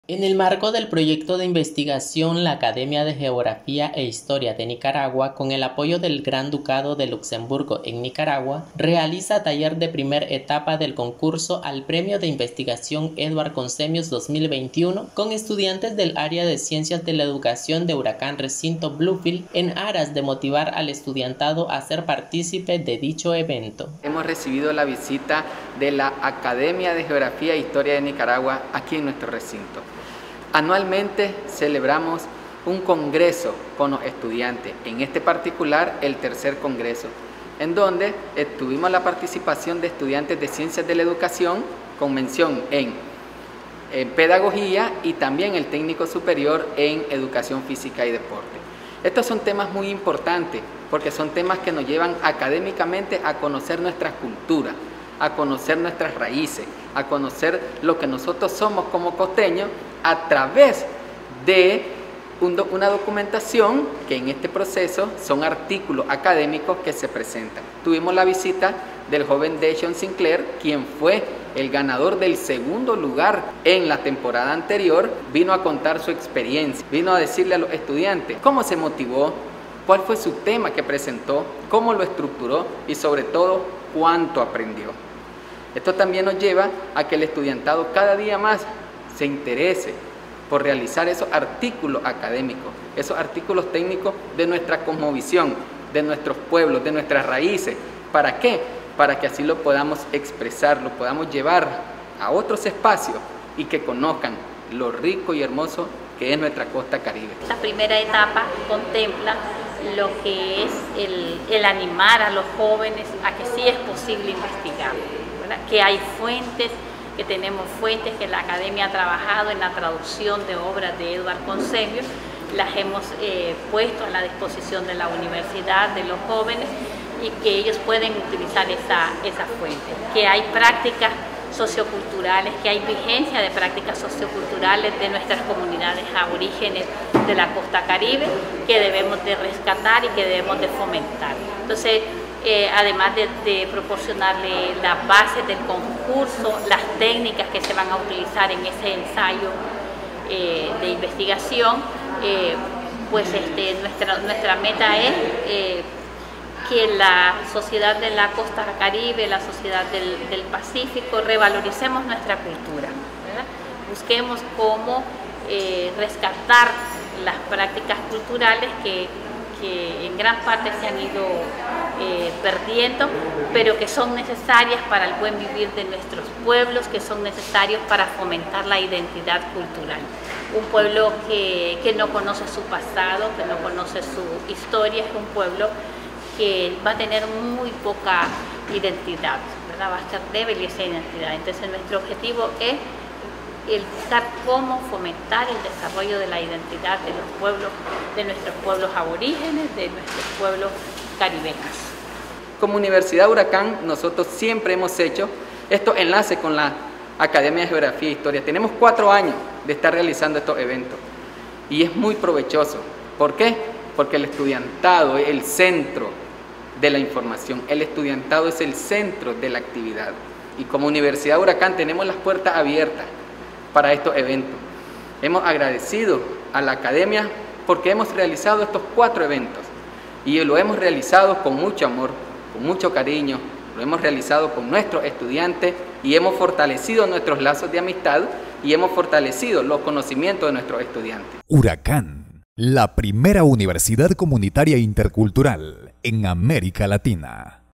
Inicio del taller “Premio a la Investigación: Eduard Conzemius 2021".
La Academia de Geografía e Historia de Nicaragua, con el apoyo del El Gran Ducado de Luxemburgo, realiza el taller de primera etapa al “Premio a la Investigación: Eduard Conzemius 2021”, con el fin de motivar a la comunidad estudiantil de URACCAN. Dichos temas de investigación son de vital importancia para el conocimiento de nuestras culturas y raíces caribeñas.